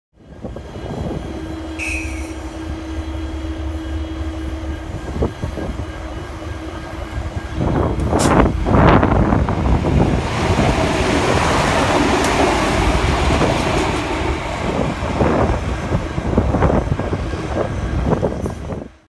Adelaide Metro Railcars passing GWA Copper Train at Greenfields
Poxboxes 3101 & 3102 are seen shortly after departing Greenfields and approaching Mawson Lakes as they work a late afternoon service from Gawler Central as a GWA copper Train (1911S with CLP14 & GWA004) waits to leave Dry Creek on Sunday 29th of October 2017